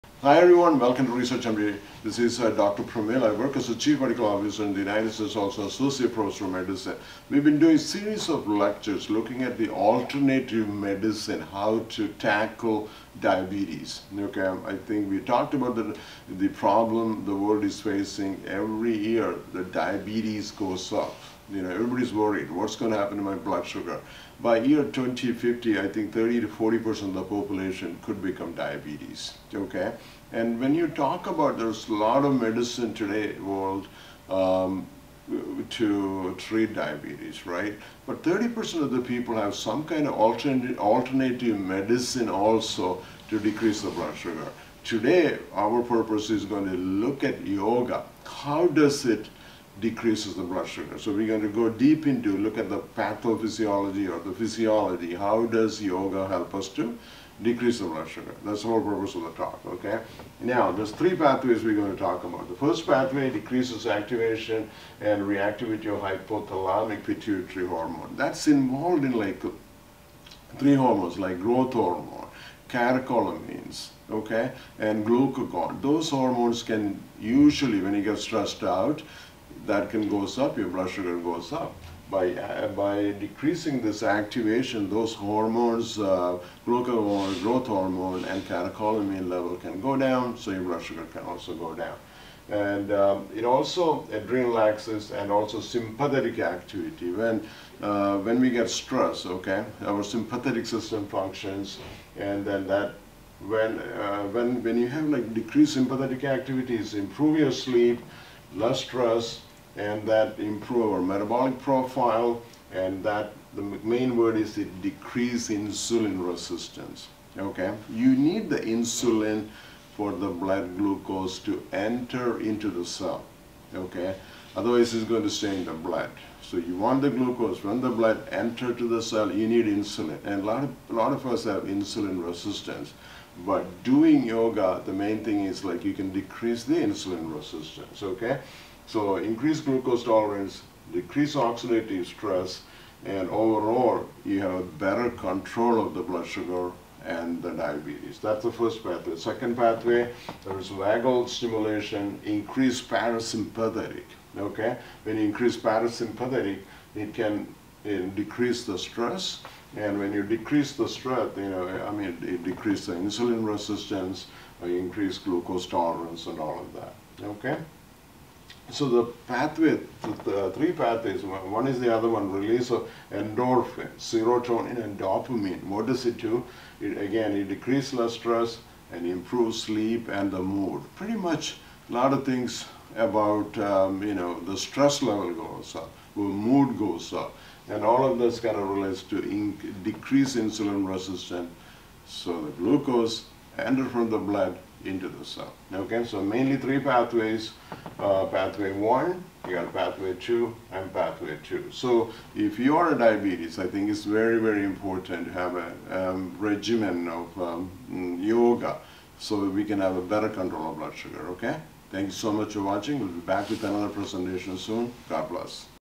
How does Yoga help to control Diabetes?
Yoga originated in India more than 5,000 years ago and is a means of balancing and harmonizing the body, mind, and emotions. Yoga practice is useful in the management of various lifestyle diseases, including type 2 diabetes.

"This video serves purely educational purposes and is not intended as medical advice; consult a healthcare professional for personalized guidance."